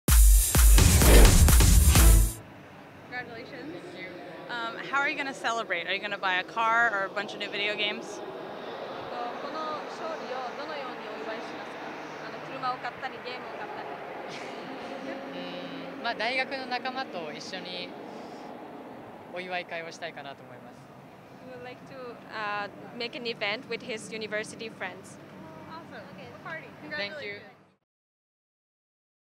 Kenzo Shirai (JPN) Interview - 2017 World Championships - Floor Final
Kenzo Shirai (JPN) Interview at the 2017 World Championships in Montreal after Event Finals day 1.